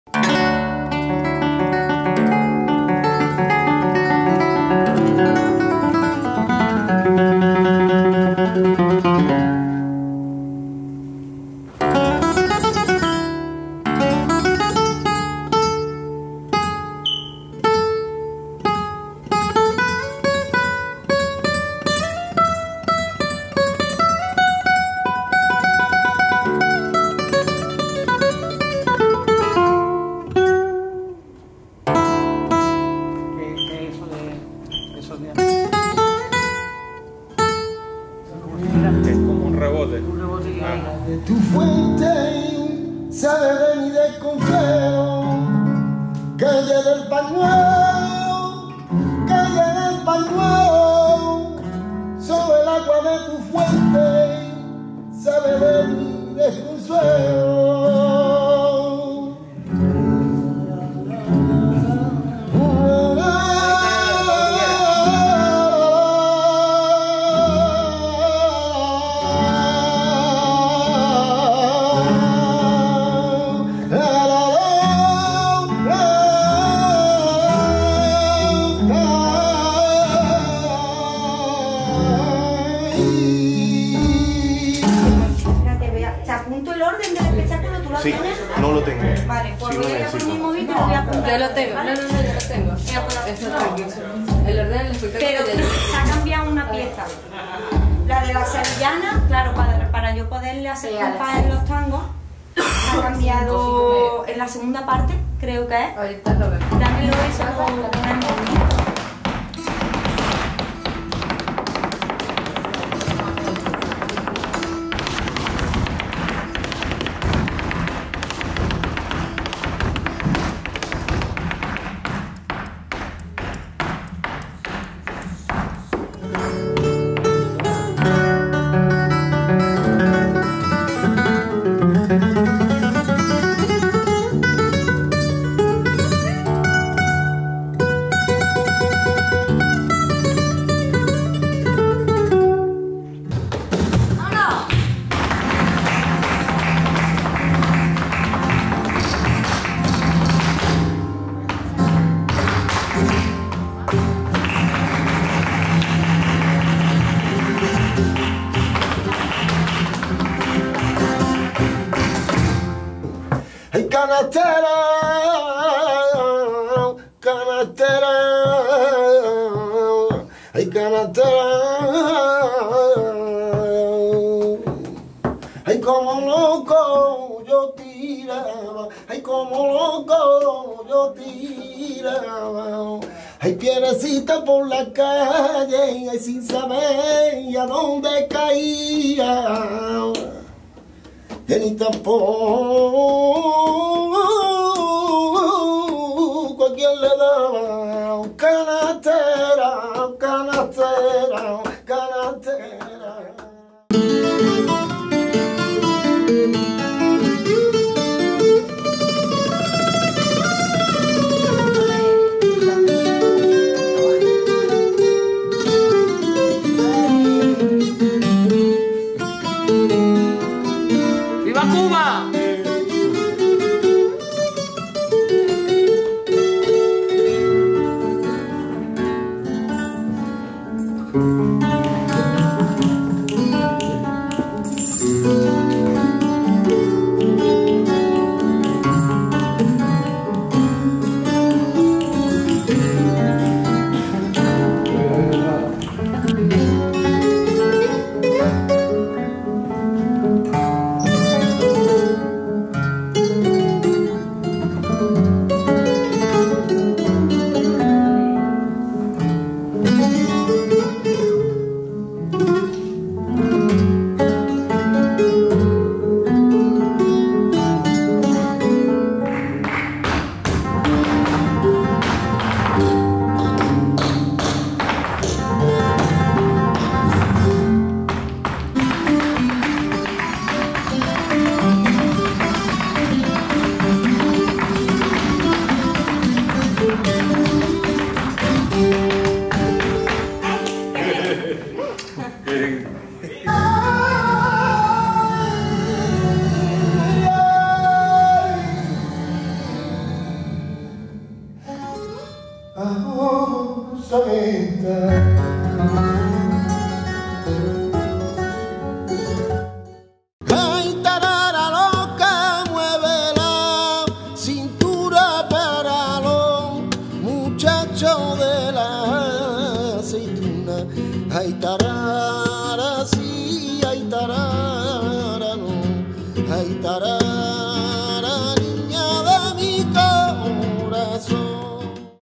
Calle del Pañuelo - Ensayos Flamencos
Una mirada íntima y personal a los ensayos del espectáculo flamenco "La Calle del Pañuelo". Realizado en el teatro en círculo en Panamá, Ciudad de Panamá. Un evento producido por Folk Lab Studio.

Con la presentación de:

Ángel Ruíz
Rubén Márquez
Laureana Granados
Mayito Echevarria

Filmado y Dirigido por: Efraín Alvarez